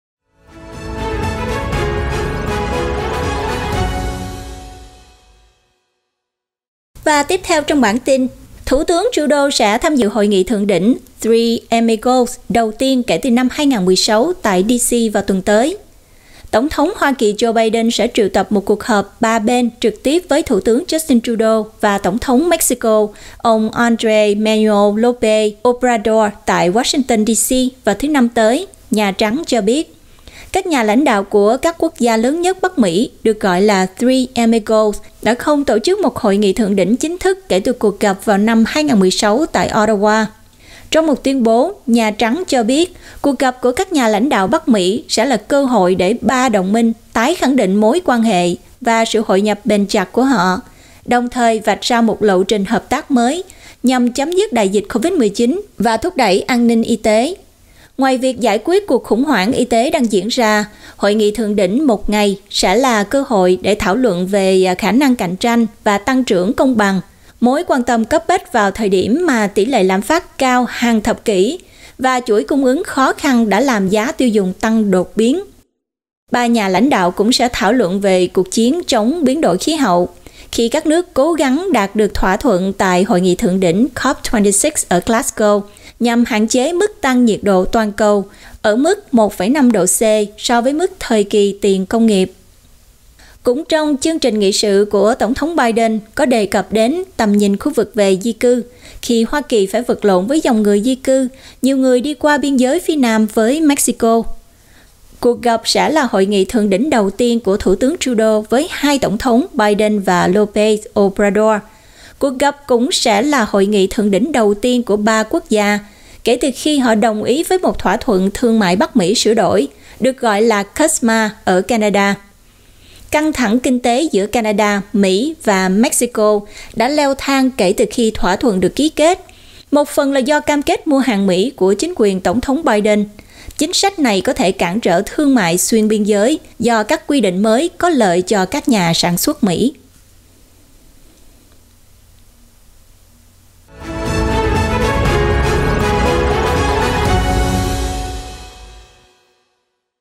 TIN CANADA | TT Trudeau sẽ tham dự Three Amigos đầu tiên kể từ năm 2016 tại D C
Kênh Culture cập nhật tin tức Canada và Thế giới hàng ngày, và xin phép nghĩ vào những ngày Lễ.